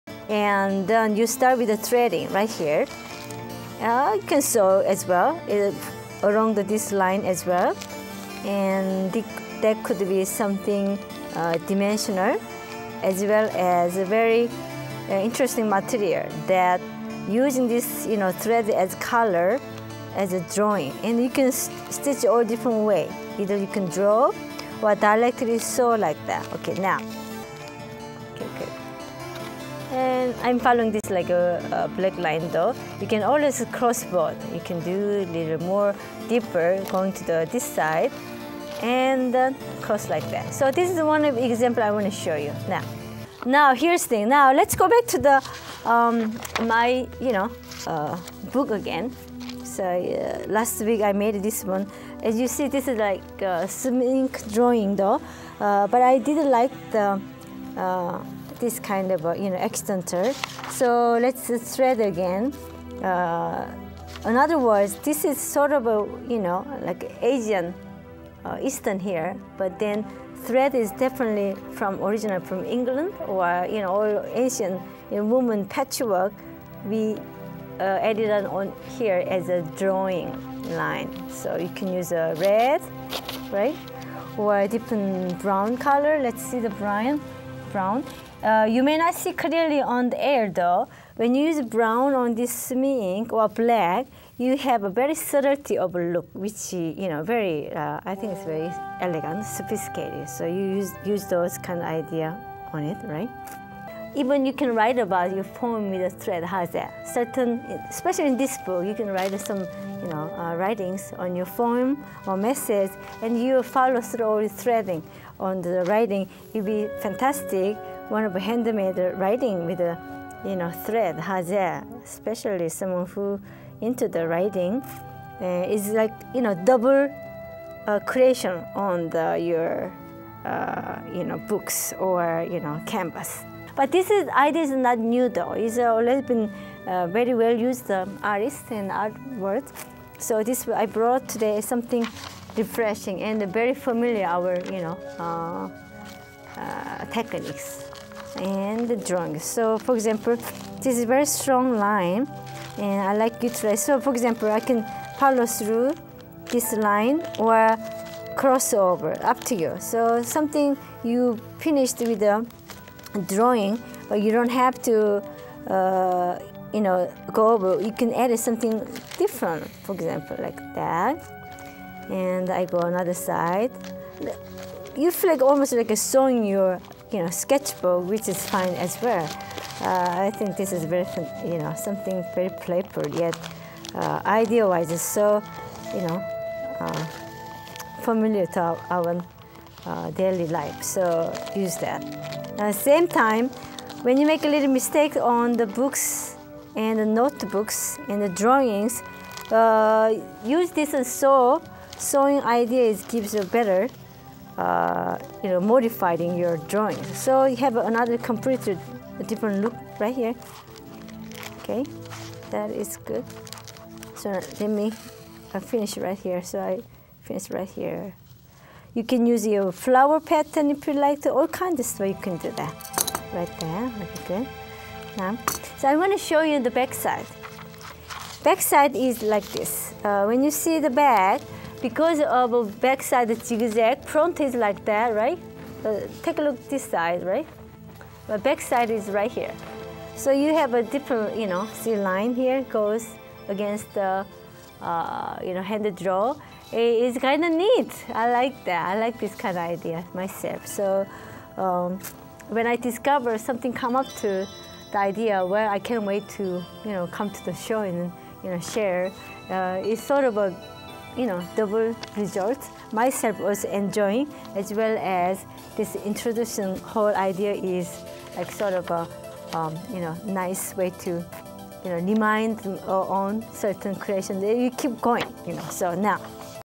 Tread sketch and sew the drawing
Combined book art of tread, stitch and drawing.
Sewing as drawing line and tread sketch and stitch drawing on sumi ink art book.